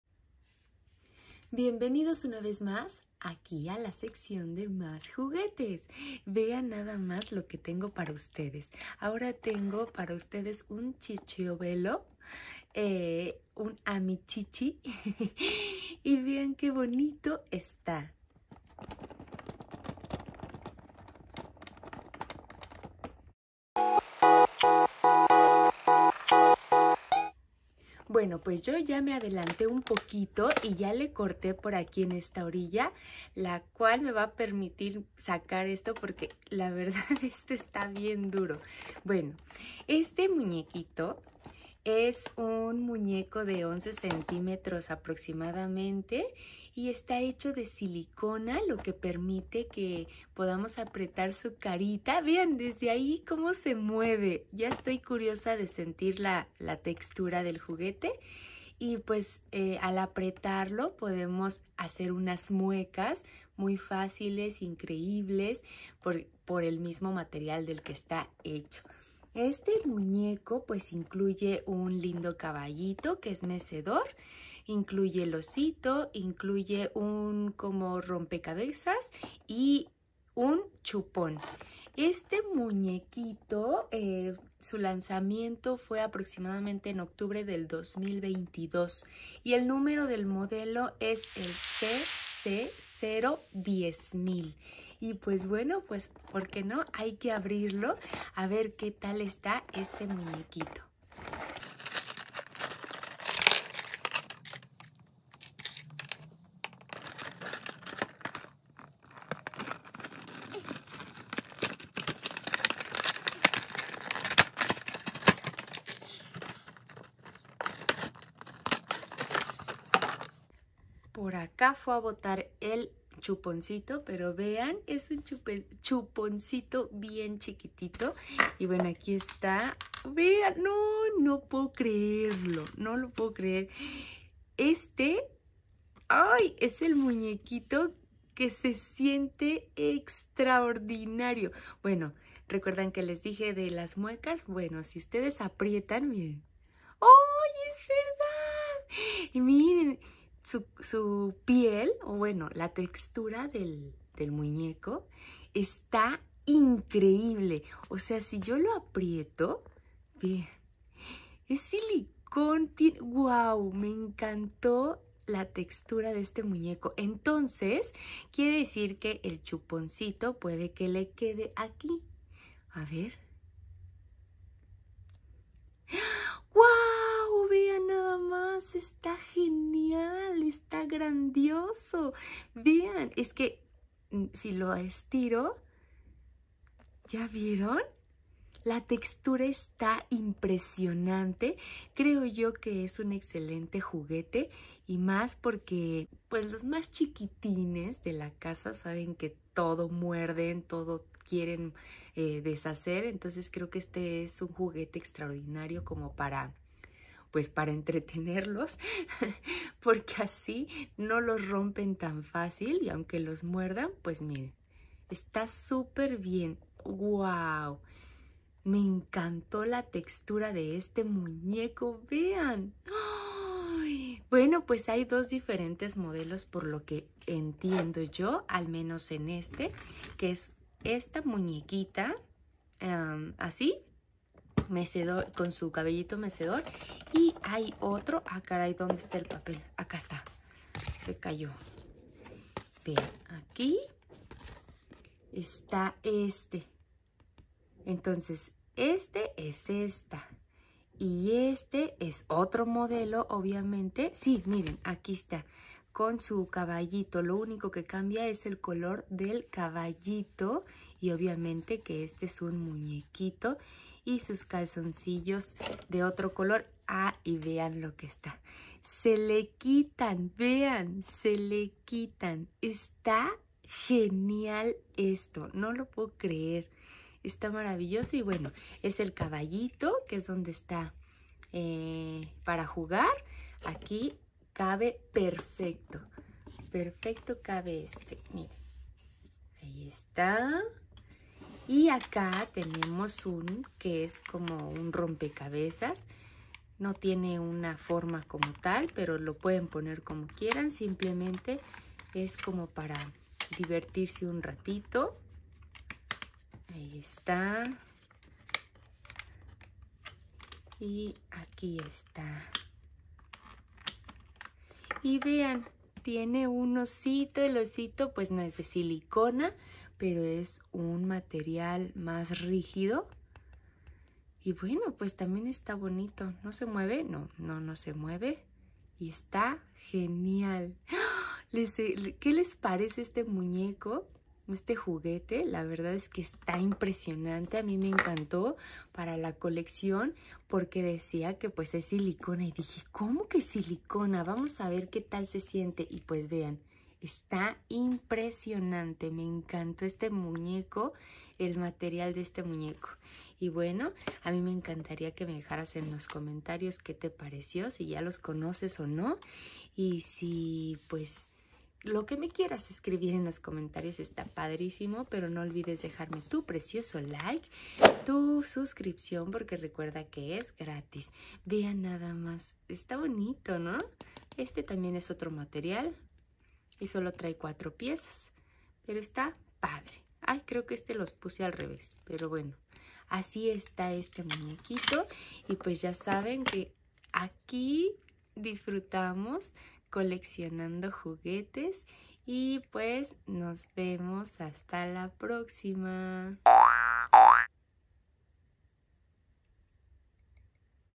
Sensacional Amicicci Cicciobello Caballito mecedor Rosa con blanco | +juguetes para coleccionar
Les comparto este cicciobello de silicon un bello amicicci excelente para la coleccion de más juguetes además la textura de ese muñeco está genial.

👶COMENTARIO
👶SUSCRIPCIÓN
👶Y COMPARTIR CON TUS AMIGOS

GRACIAS!!!